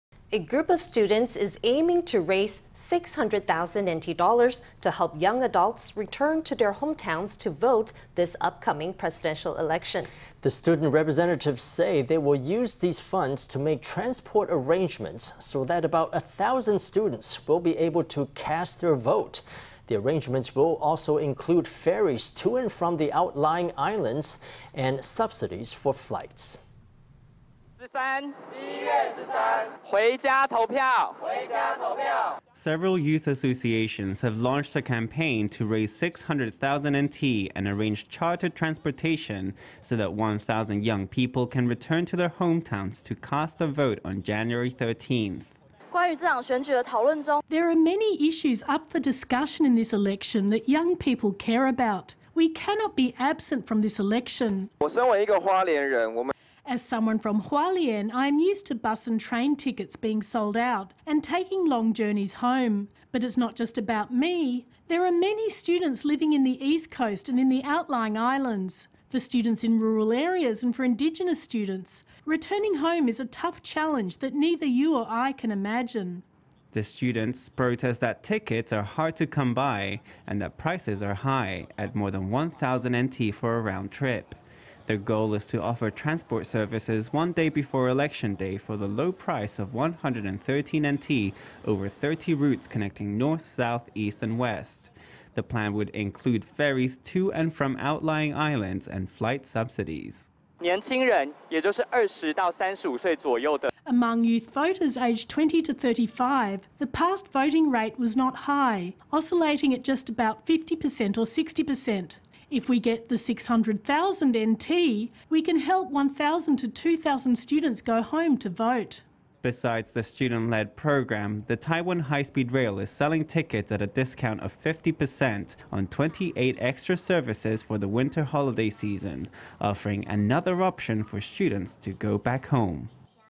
Students launch fundraiser to help young adults go home to vote on Jan. 13｜Taiwan News
A group of students is aiming to raise NT$600,000 to help young adults return to their hometowns to vote this upcoming presidential election. The student representatives say they will use these funds to make transport arrangements so that about 1,000 students will be able to cast their vote. The arrangements will also include ferries to and from the outlying islands and subsidies for flights.

Several youth associations have launched a campaign to raise NT$600,000 and arrange chartered transportation so that 1,000 young people can return to their hometowns to cast a vote on Jan. 13.

Chen Yi-chin
Student representative
There are many issues up for discussion in this election that young people care about. We cannot be absent from this election.

Sera Fangis Pacidal
Student representative
As someone from Hualien, I am used to bus and train tickets being sold out and taking long journeys home. But it’s not just about me. There are many students living in the east coast and in the outlying islands. For students in rural areas and for Indigenous students, returning home is a tough challenge that neither you nor I can imagine.

The students protest that tickets are hard to come by and that prices are high, at more than NT$1,000 for a round trip. Their goal is to offer transport services one day before Election Day for the low price of NT$113 over 30 routes connecting north, south, east and west. The plan would include ferries to and from outlying islands and flight subsidies.

Chang Yu-meng
Taiwan Youth Association for Democracy
Among youth voters aged 20 to 35,the past voting rate was not high, oscillating at just about 50% or 60%. If we get the NT$600,000, we can help 1,000 to 2,000 students go home to vote.

Besides the student-led program, the Taiwan High Speed Rail is selling tickets at a discount of 50% on 28 extra services for the winter holiday season, offering another option for students to go back home.